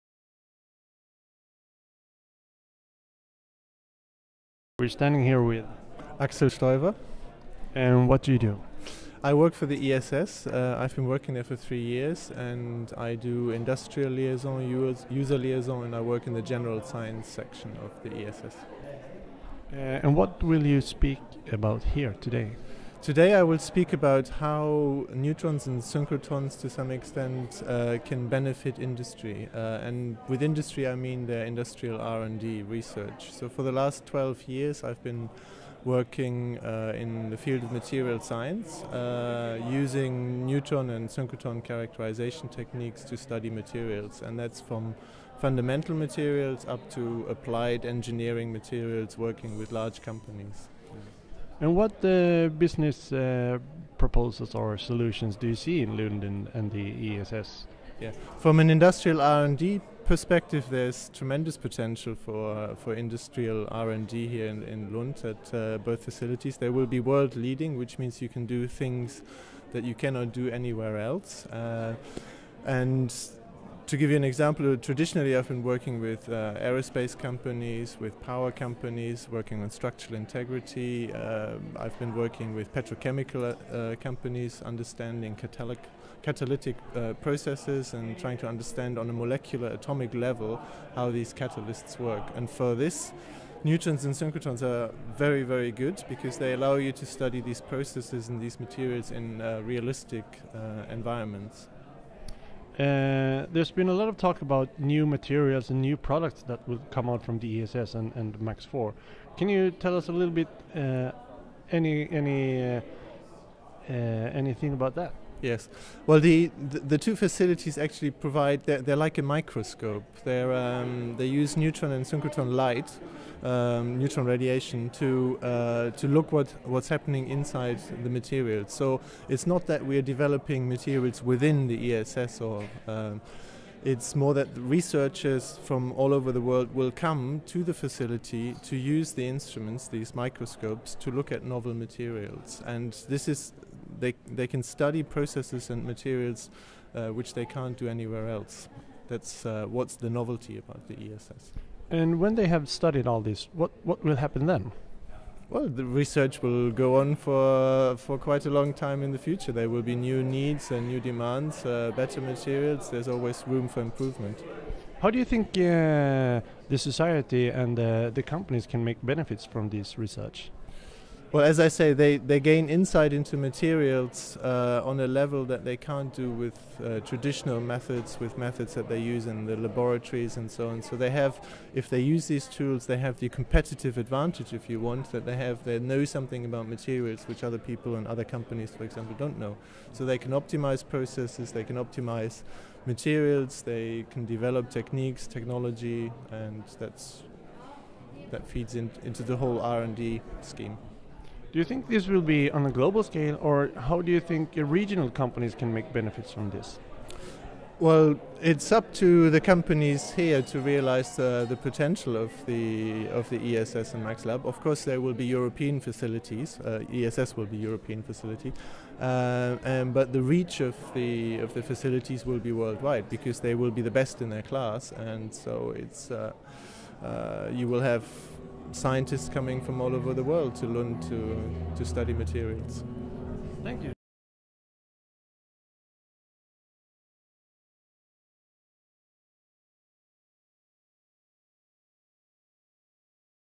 ESS MAX IV i regionen - TITA, 2011
Konferens i Malmö 1 april 2011 intervju med föredragshållaren Axel Steuwer, Expert Researcher, ESS